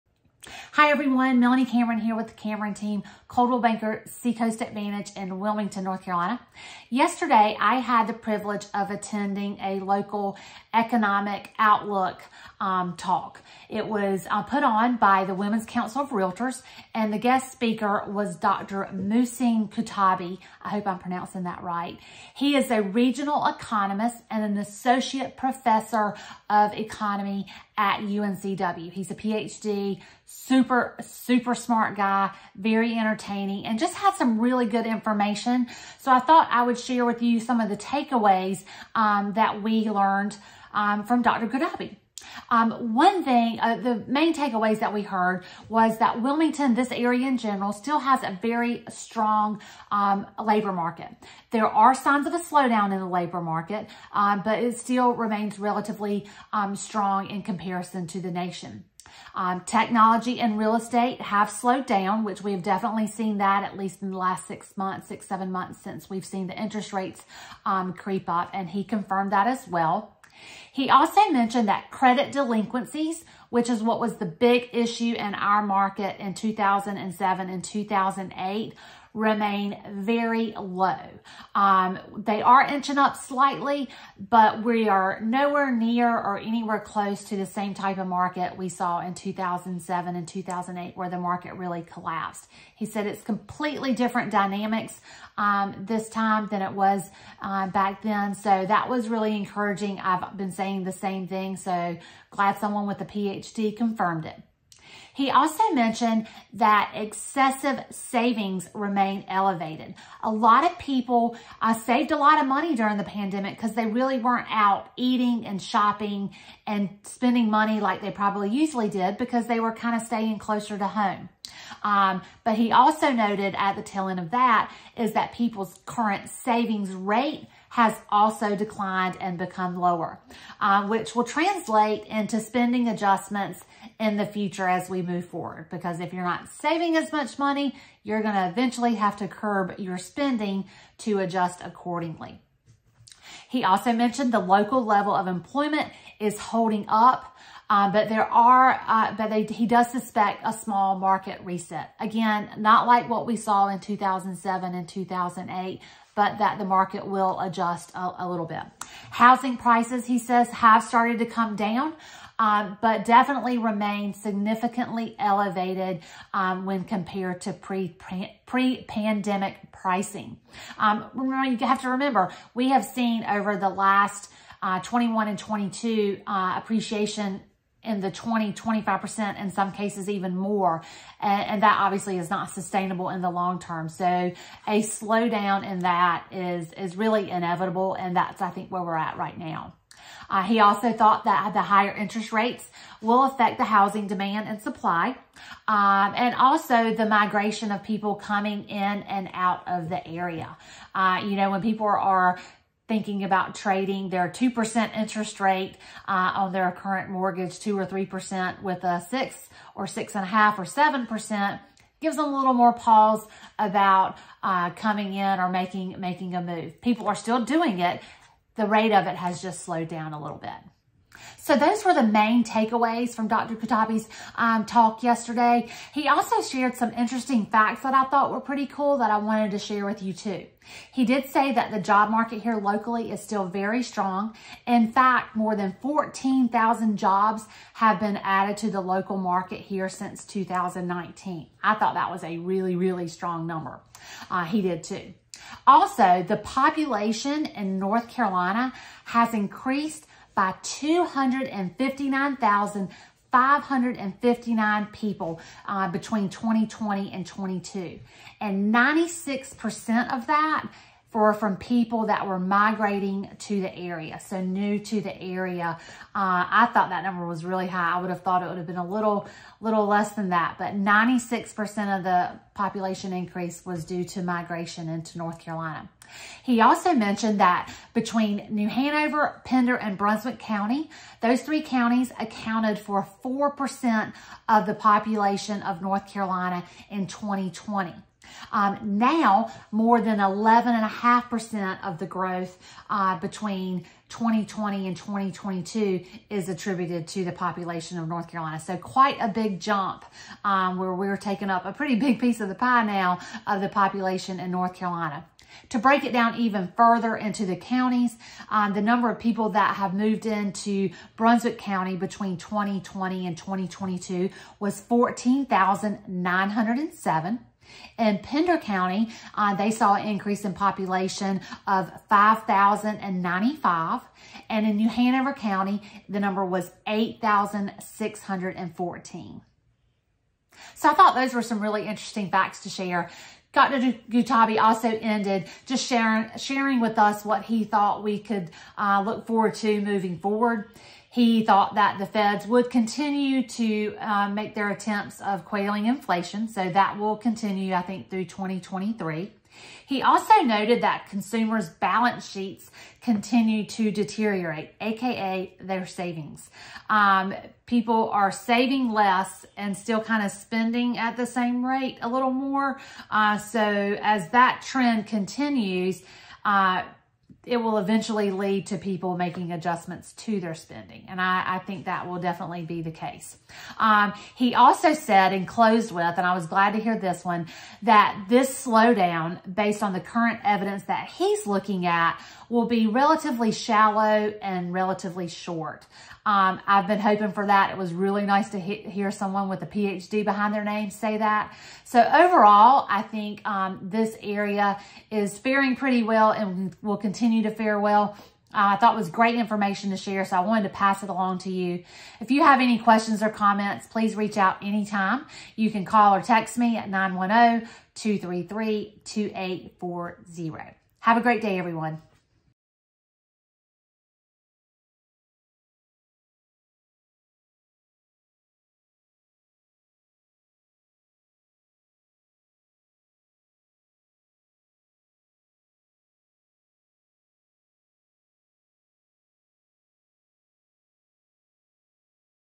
Wilmington NC Economic Outlook
Realtor Melanie Cameron shares highlights from the Wilmington Economic Outlook presentation by Dr. Mouhcine Guetta, an Associate Professor of Economics at UNCW.  This highly interesting and detailed presentation included important stats about the Wilmington, NC, economy, including the population growth we've experienced in recent years, the health of our current local economy, and what we can expect for the rest of 2023 and beyond. Will Wilmington be as hard hit as other areas? Watch the video to find out.

Melanie Cameron, Broker
The Cameron Team
Coldwell Banker Sea Coast Advantage